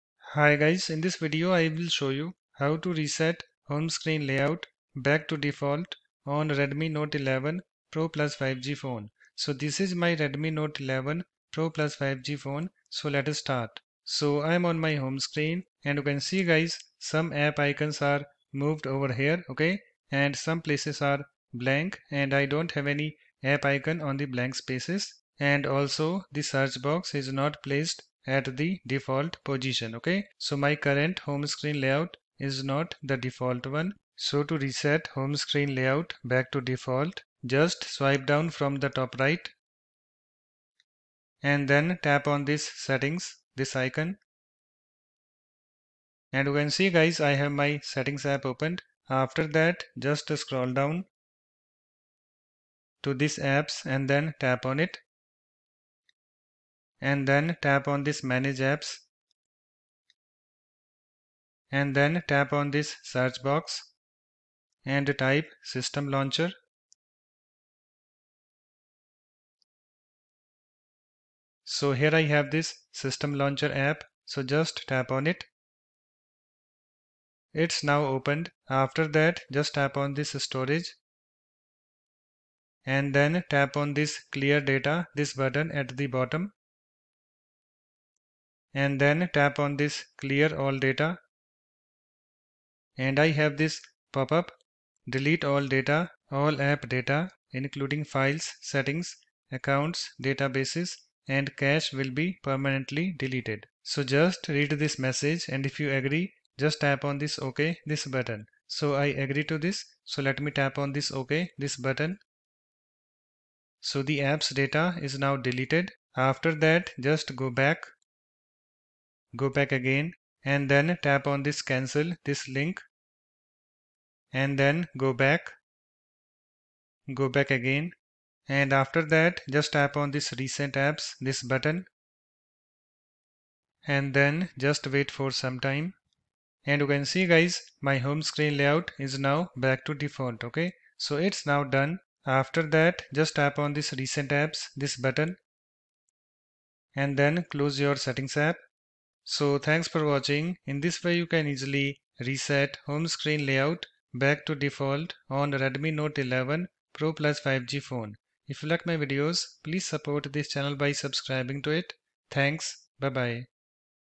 How to Reset Home Screen Layout Back to Default On Redmi Note 11 Pro+ 5G Phone?
Hi guys, In this Video, I will show you "How to Reset Home Screen Layout Back to Default On Redmi Note 11 Pro+ 5G Phone."